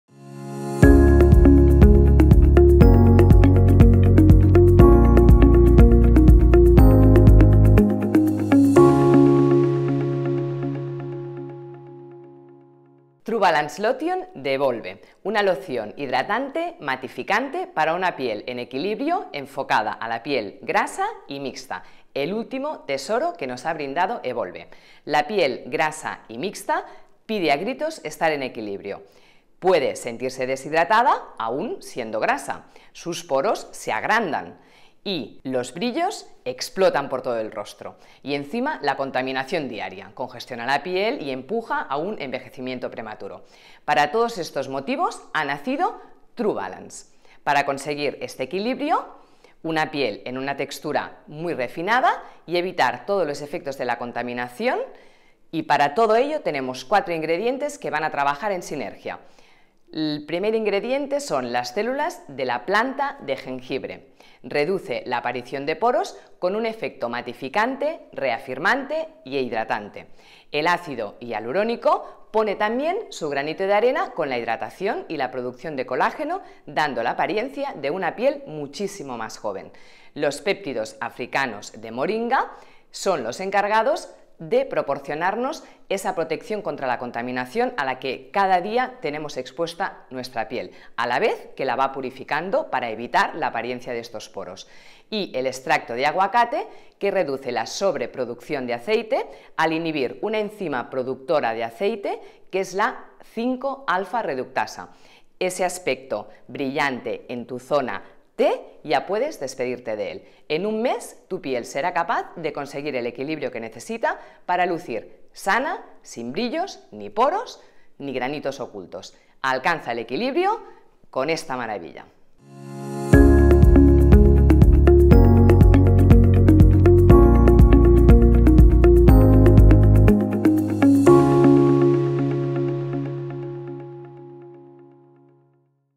True Balance Lotion | Hidratante antiedad Matificante
Una solución hidratante matificante antiedad para pieles mixtas y grasas. Olvídate de brillos, poros abiertos y disfruta de una piel equilibrada. Dale a tu piel lo que necesita y no te arruinará el día!